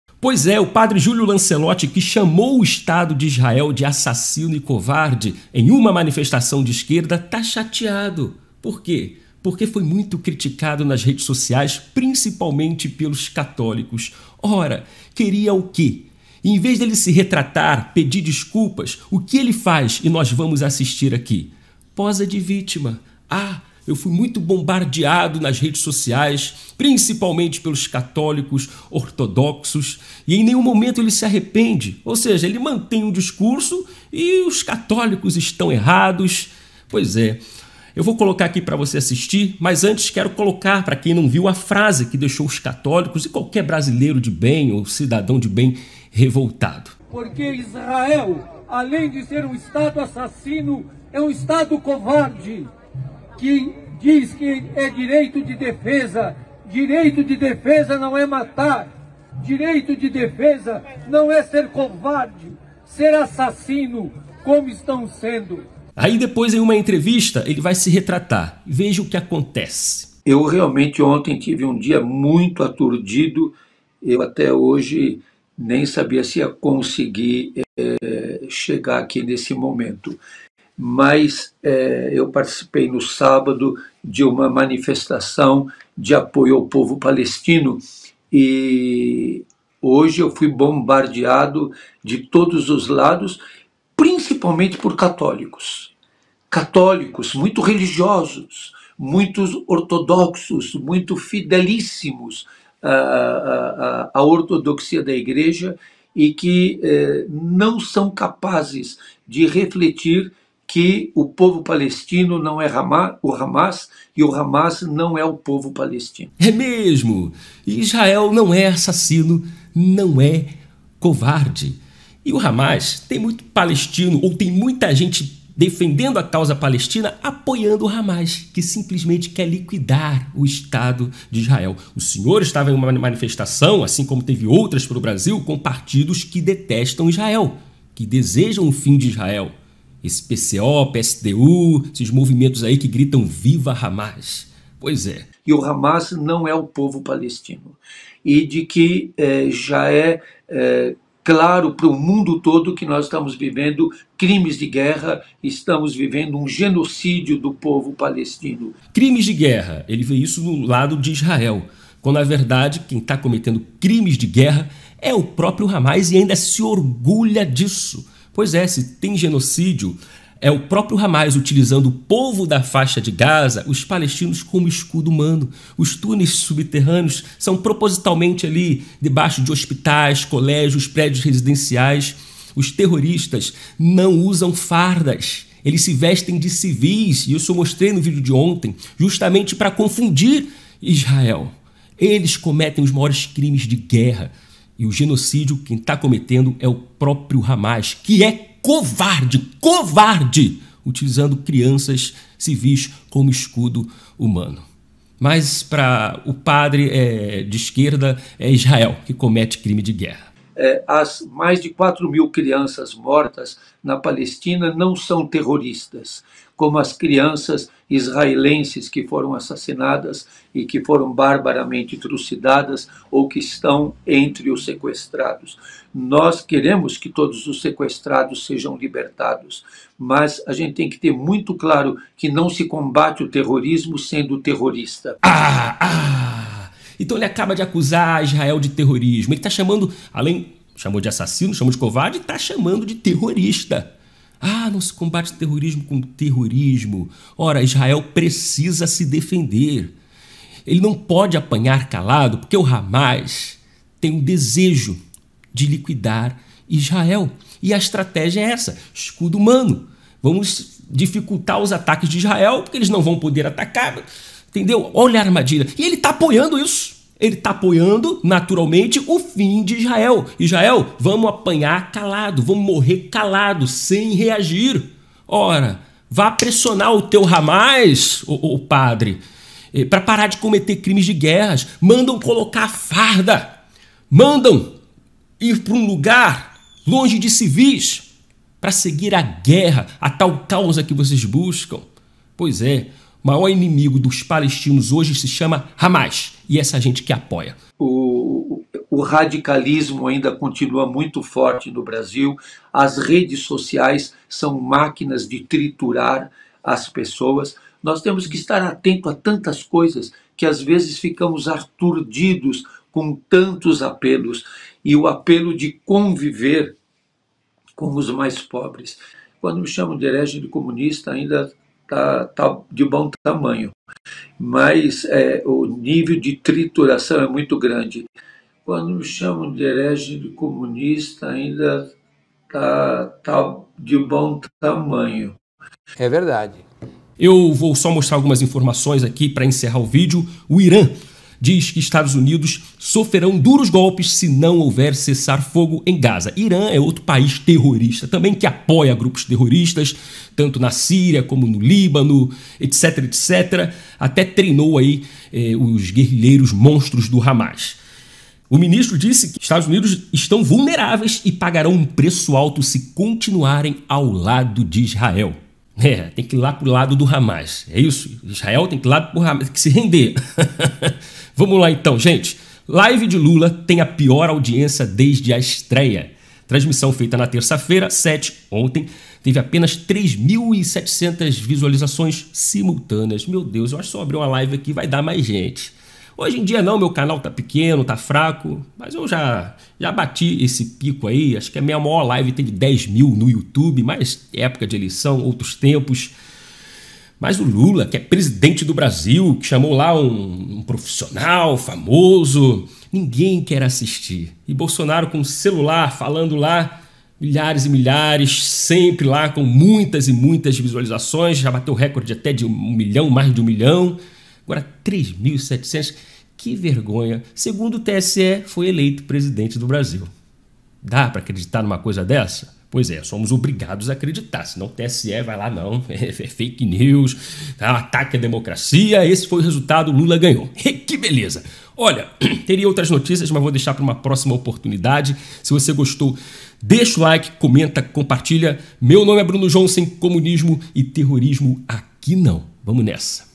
Padre Júlio Lancellotti posou de vítima após ser criticado nas redes / A pior live do Lula
Agência: 0889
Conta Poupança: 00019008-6
Bruno Augusto Jonssen